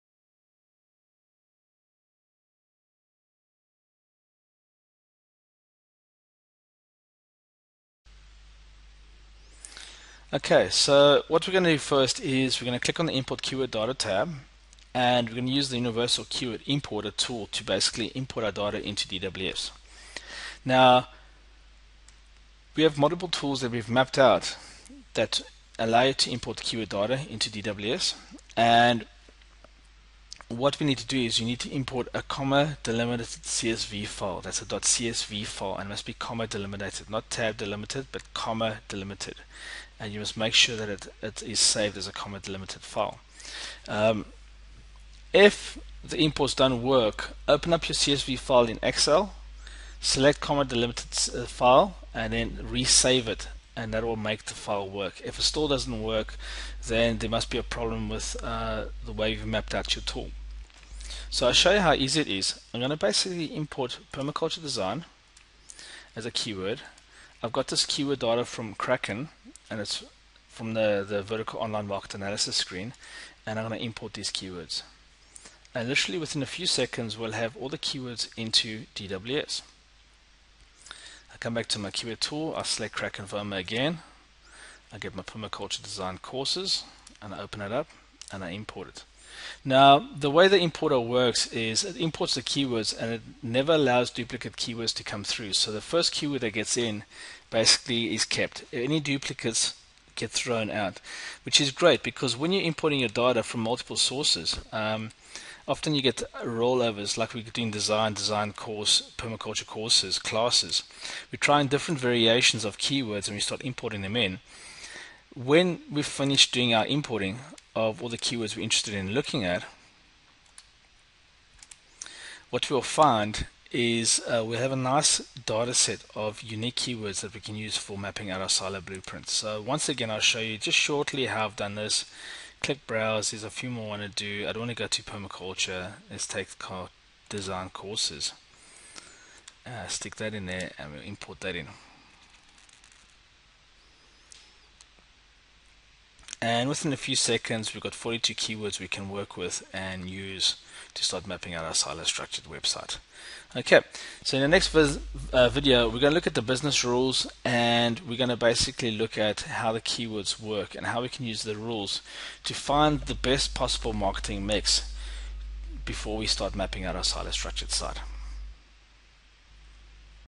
5: How do you import keywords into Domain Web Studio
Domain Web Studio gives you the ability to use any keyword source as long as the source uses phrase match competing pages and search volume. The process super easy and in this video we show you how quickly this step is done.